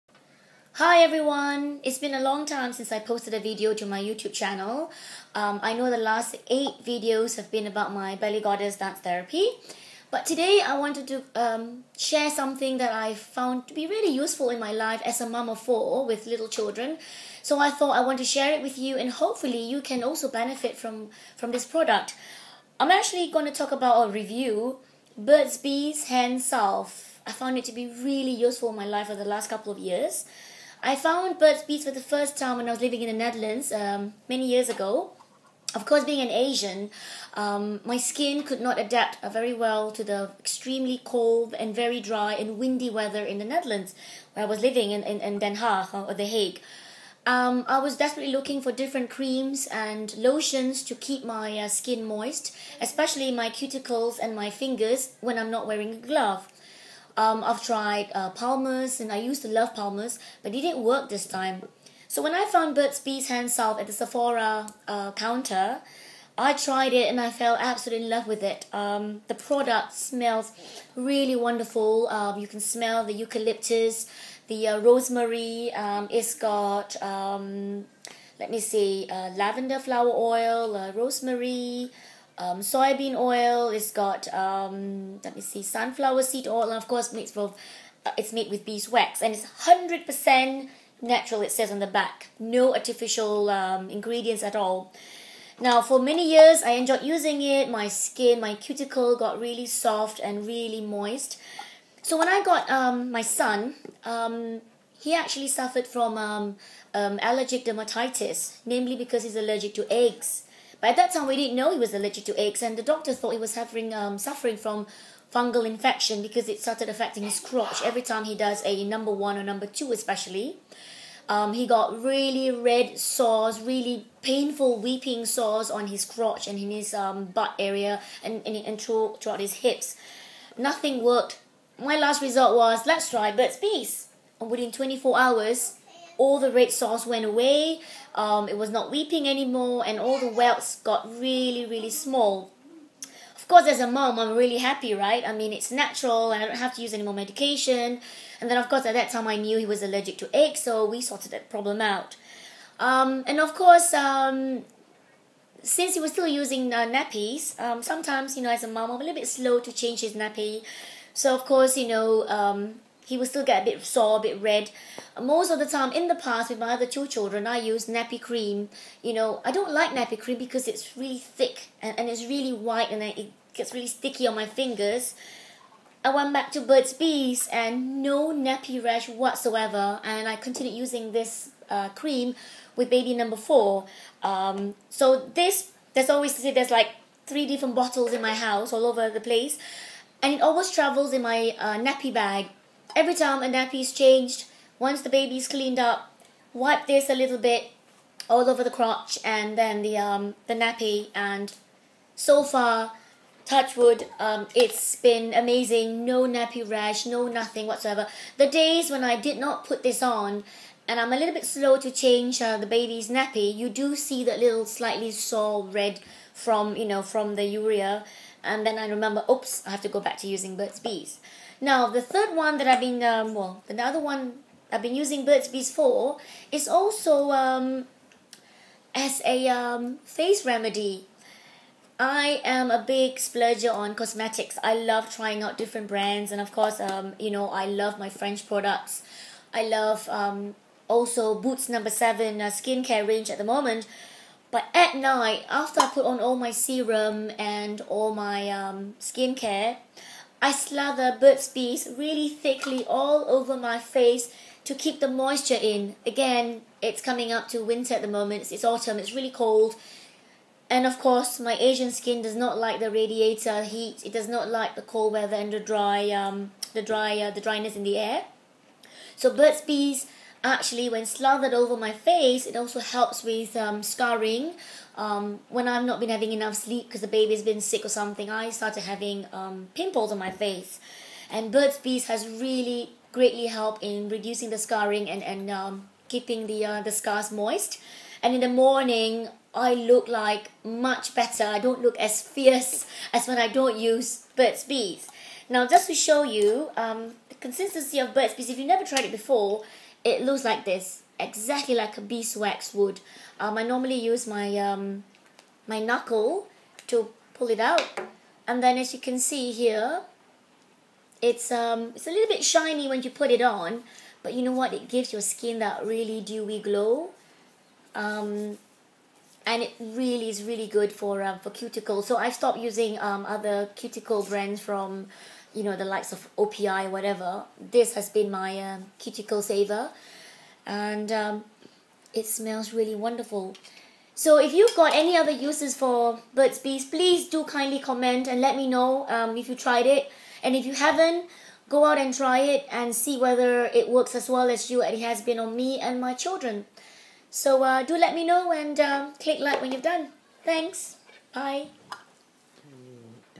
Review of Burt's Bees Hand Salve used as Nappy Cream, Face Saver and Dermatitis cream.
After a long time away, I decided to do a video of one of my favourite household product. We use it for many many things and I hope you find your own use for it. Share your own amazing products and I'll try that too!